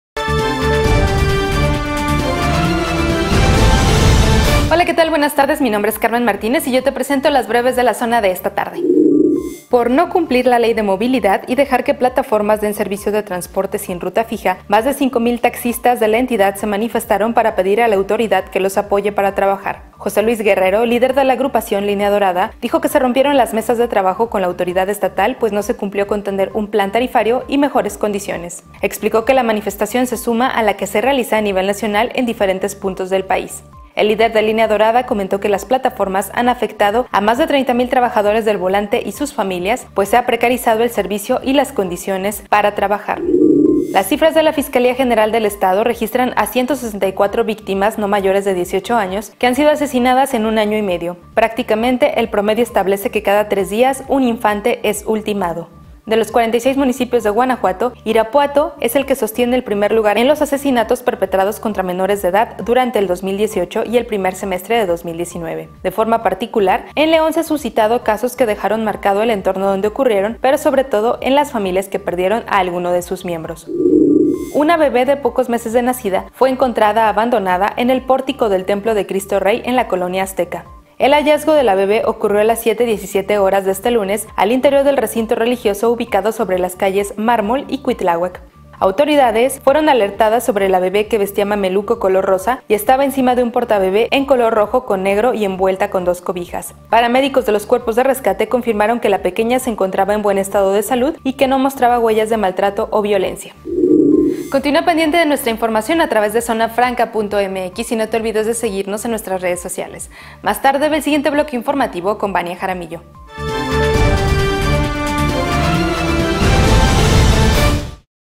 Taxistas se manifiestan contra plataformas móviles en León | Breves de la Zona
No te pierdas LoÚltimo que pasa en Guanajuato con las BrevesDeLaZona📰💻 de Carmen Martínez
- Taxistas se manifiestan contra plataformas móviles en León; rompen con gobierno.
- En Guanajuato, 164 niños y jóvenes han sido asesinados en año y medio.
- En León, abandonan recién nacida en iglesia.